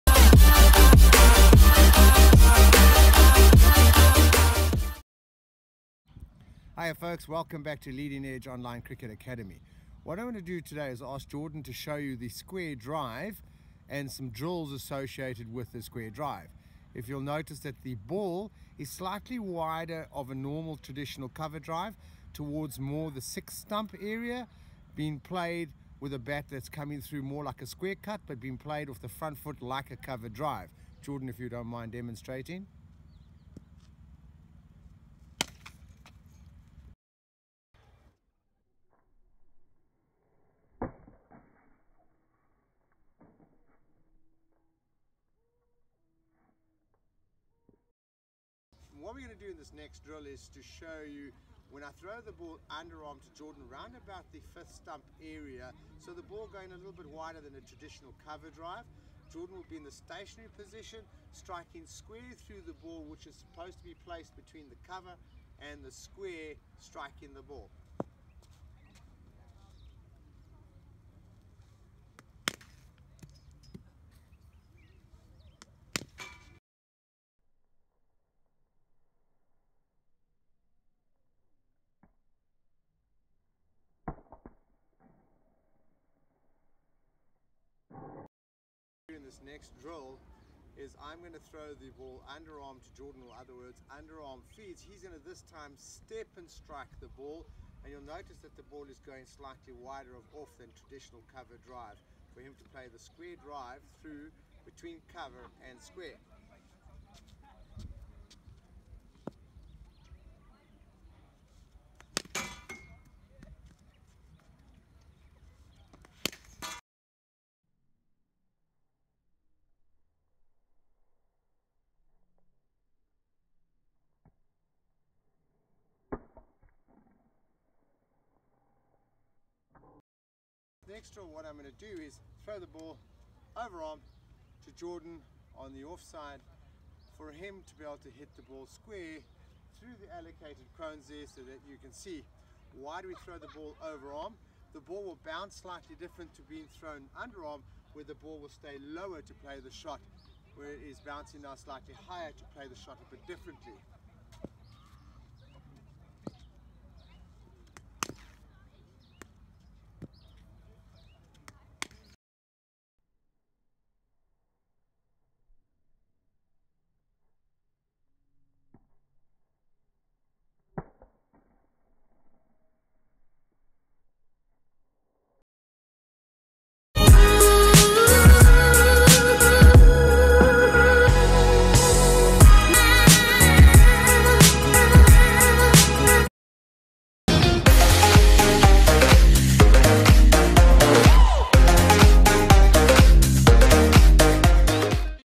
HOW TO PLAY THE SQUARE COVER DRIVE / SQUARE COVER DRIVE DRILLS
In this video we will be learning how to play the square cover drive and the best drills to use in your next net practice.